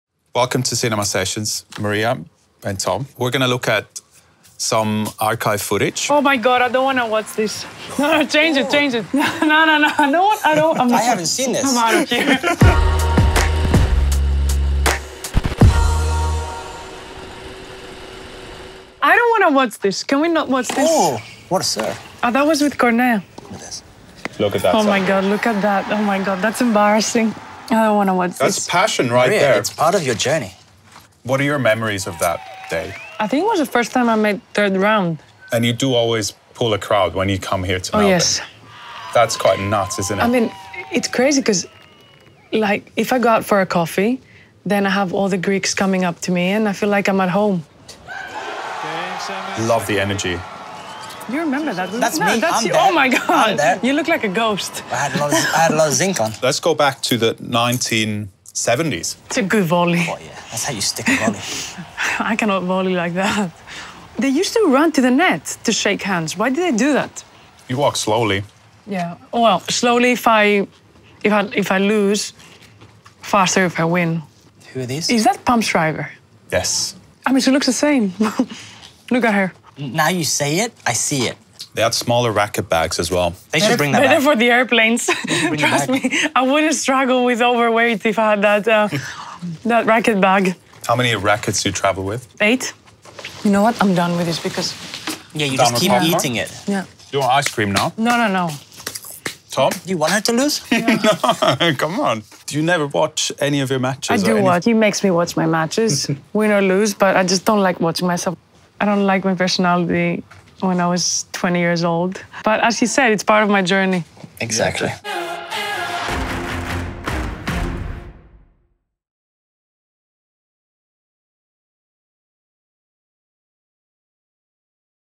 Sakkari Revisits Memorable Beginnings with 70s Tennis Icons | Cinema Sessions | Australian Open 2024
Join Maria Sakkari as she revisits her early career, and explores the timeless legacy of tennis icons from the 70s, including the remarkable Pam Shriver in a cinema session at the Australian Open 2024.